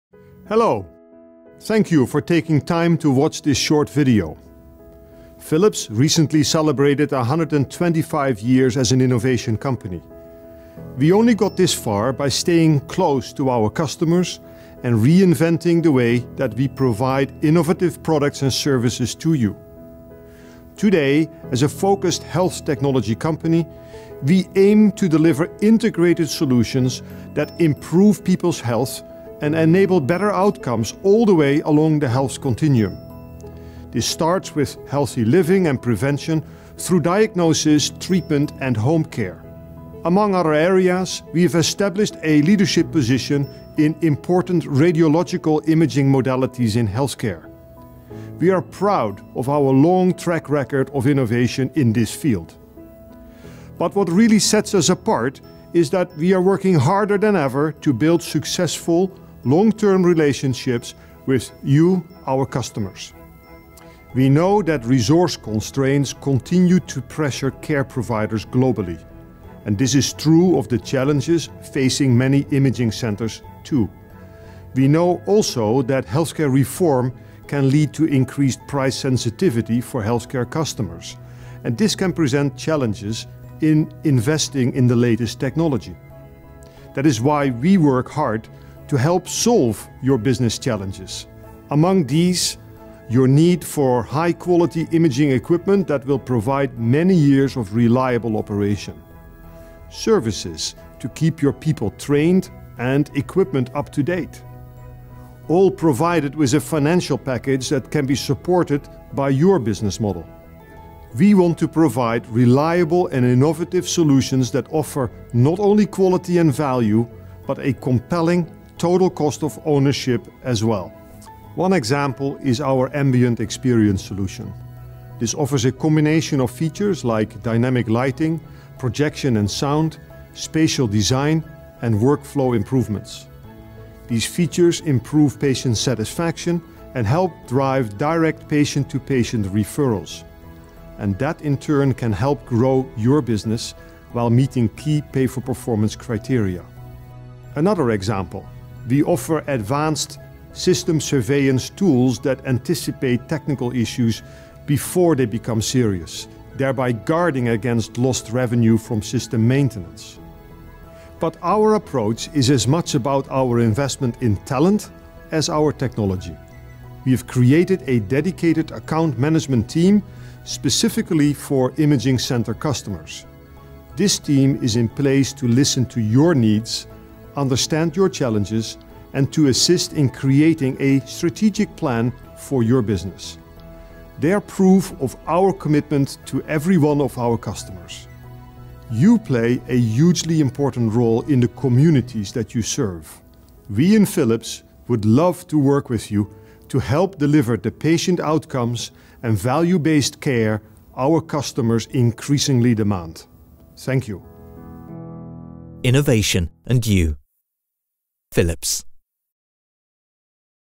Diagnostic Imaging Centers: Executive Introduction
At Philips, we are committed to the Diagnostic Imaging Centers market and understand the challenges faced there today.  Philips helps provide partners in this market segment with the best solutions to positively impact their business growth.